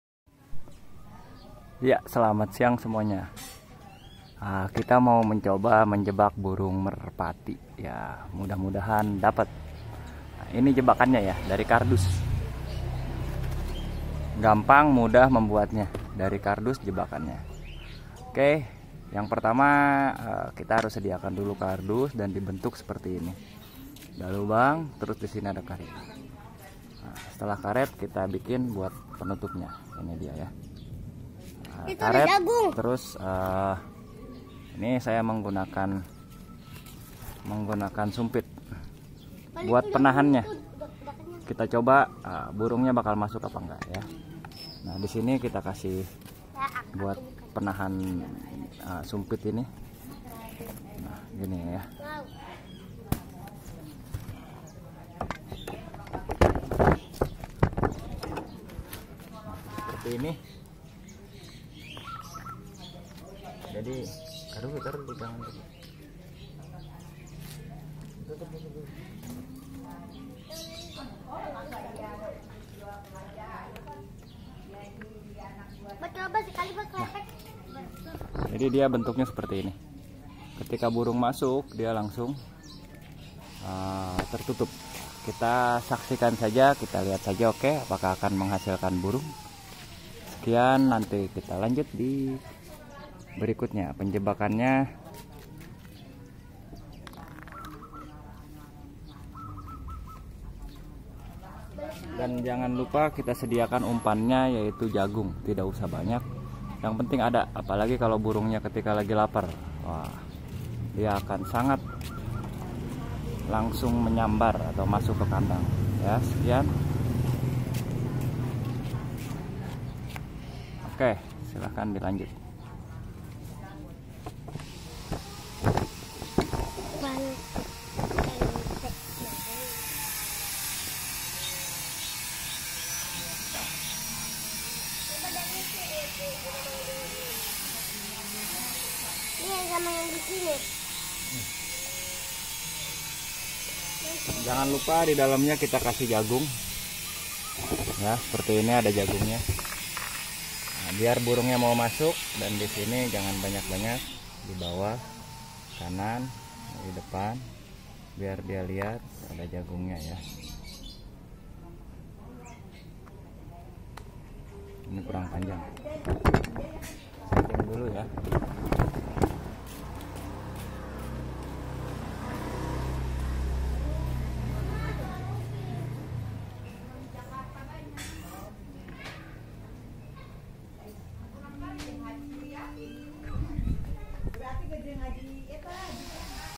Gagal nangkap burung
Menangkap burung dengan alat sederhana.  hampir dapet.